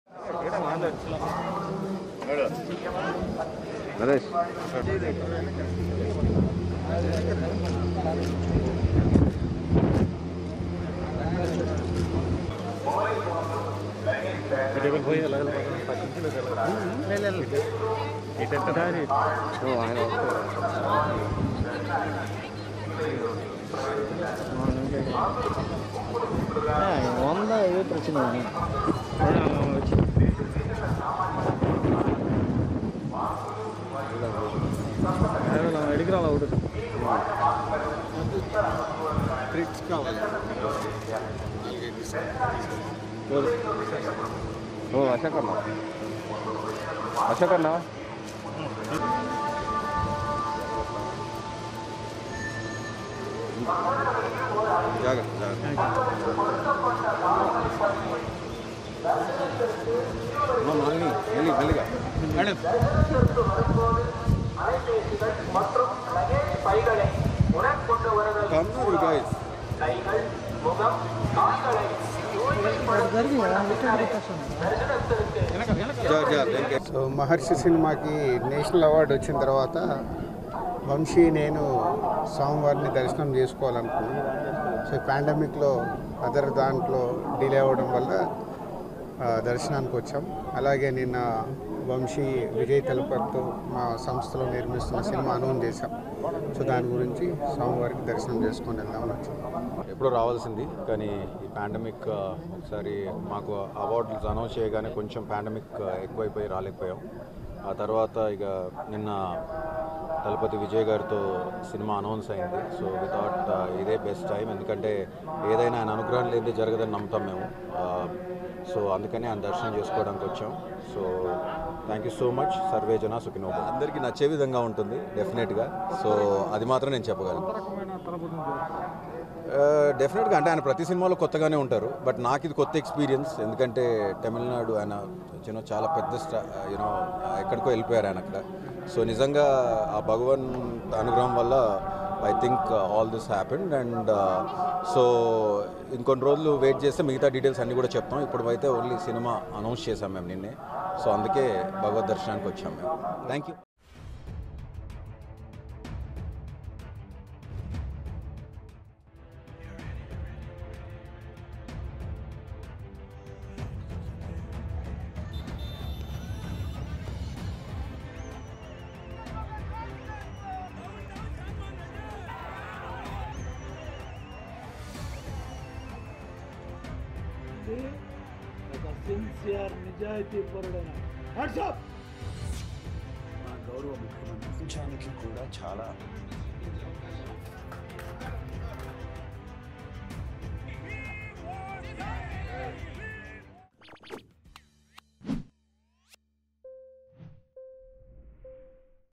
Tirumala: శ్రీవారిని దర్శించుకున్న దిల్ రాజు, వంశీ పైడిపల్లి | Dil Raju and Vamsi Paidipally
తిరుమల శ్రీవారిని ప్రముఖ దర్శక, నిర్మాతలు దర్శించుకున్నారు. ఇవాళ ఉదయం విఐపి విరామ సమయంలో ప్రముఖ సినీ నిర్మాత దిల్ రాజు, ప్రముఖ సినీ దర్శకుడు వంశీ పైడిపల్లి స్వామి వారి సేవలో పాల్గొని మొక్కులు చెల్లించుకున్నారు. దర్శన అనంతరం  రంగనాయకుల మండపంలో వేదపండితులు వేదాశీర్వచనం అందించగా ఆలయ అధికారులు స్వామి వారి తీర్థప్రసాదాలు అందజేశారు. ఆలయం వెలుపల దిల్ రాజు మాట్లాడుతూ.. మహర్షి సినిమాకు జాతీయ అవార్డు రావడంతో చిత్ర దర్శకుడు వంశీతో కలసి స్వామి వారిని దర్శించుకున్నామని అన్నారు.విజయ్ దళపతితో ఓ సినిమా వంశీ దర్శకత్వంలో నిర్మిస్తున్నమని తెలిపారు.అనంతరం వంశీ పైడిపల్లి మాట్లాడుతూ.. దిల్ రాజు ప్రొడక్షన్ లో విజయ్ దళపతితో సినిమా సినిమా చిత్రీకరిస్తున్నమని తెలిపారు. శ్రీవారి అనుగ్రహం కోసం తిరుమలకు వచ్చామన్నారు.అందరికి నచ్చేలా సినిమా ఉంటుందని పేర్కొన్నారు.